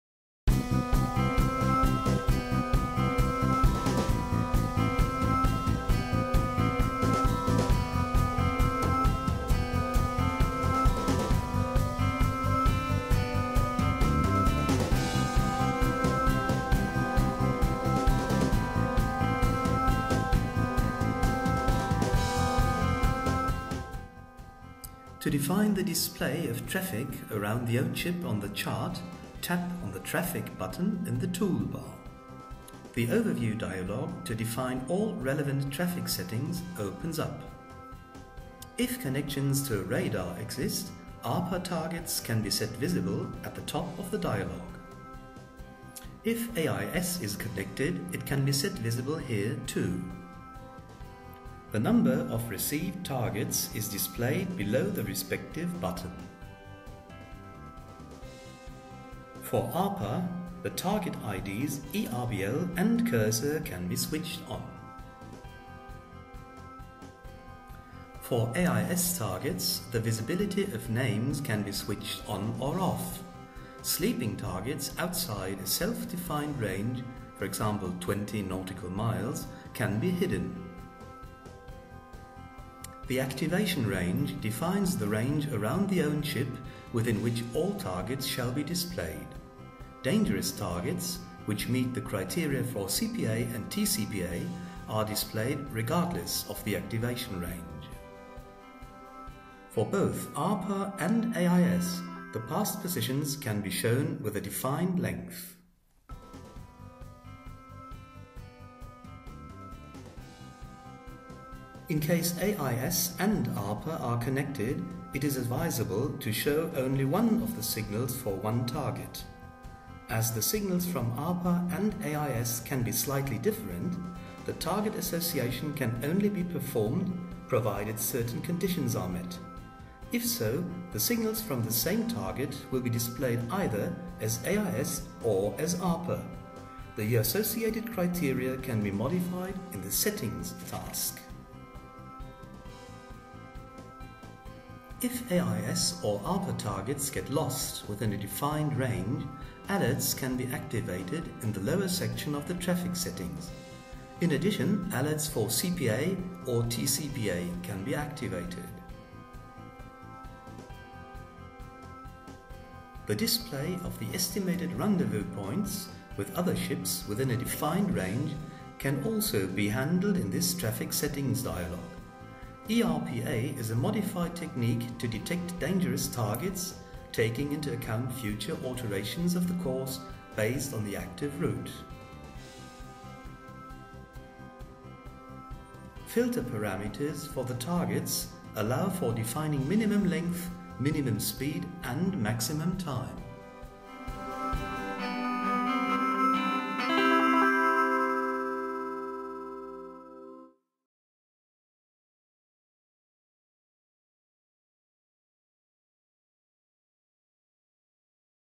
Traffic Settings and Sensor Management
The Traffic button opens a window with traffic related settings like display of ARPA and/or AIS targets, alerts and ERP.